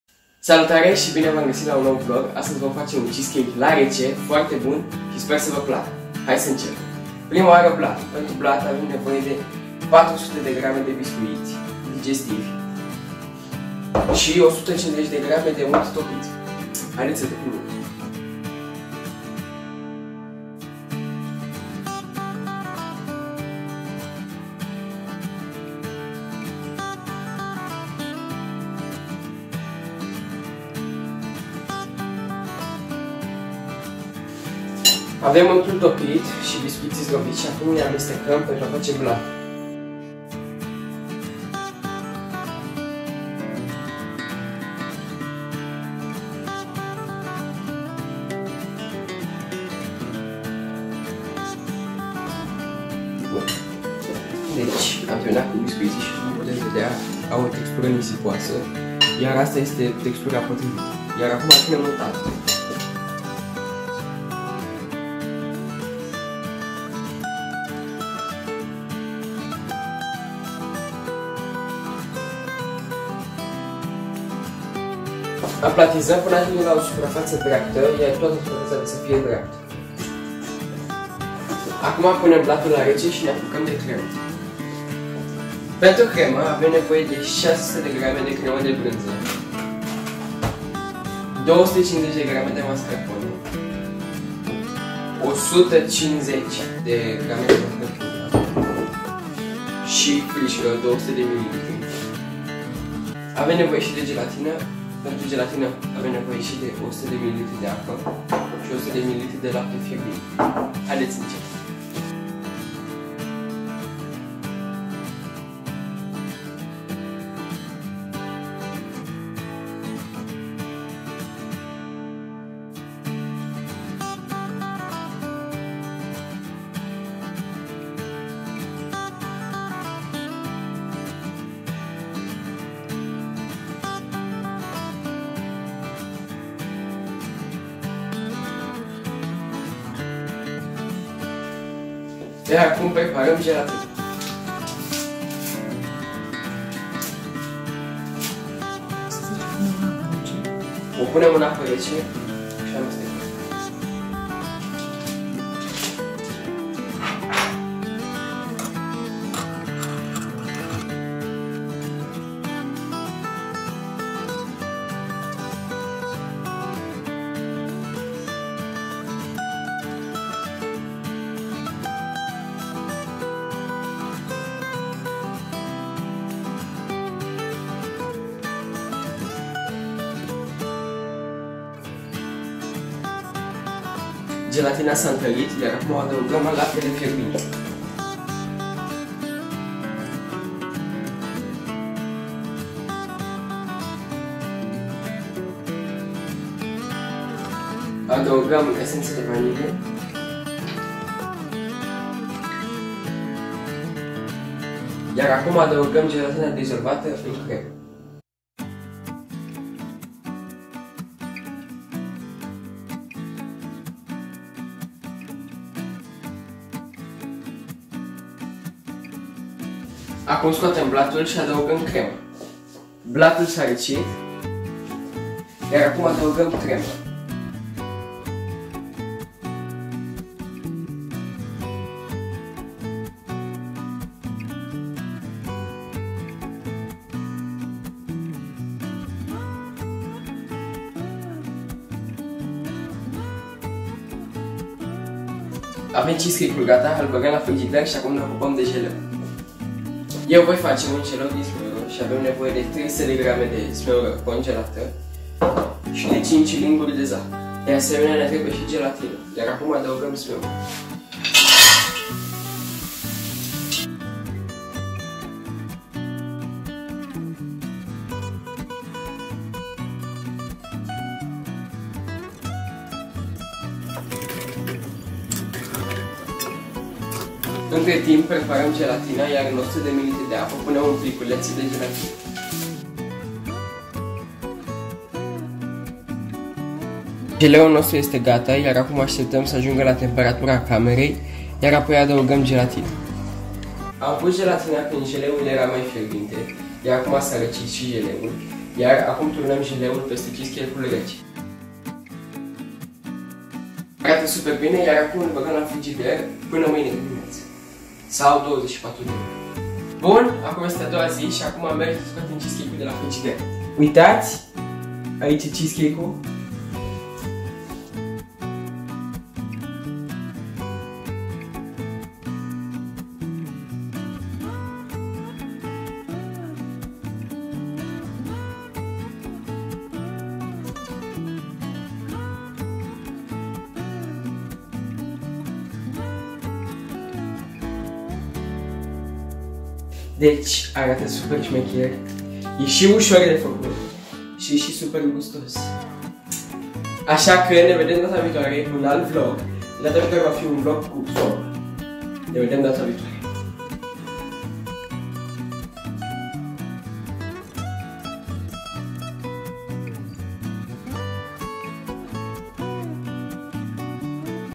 Am facut CHEESECAKE super gustos fara coacere | How to Make the Creamiest Cheesecake
Salutare si bine v-am gasit la un nou video. ,astazi am facut un cheesecake super fain si super gustosss!!! Sper sa va placa si sa il preparati si voi!!!

Am facut un CHEESECAKE super gustos | How to Make the Creamiest Cheesecake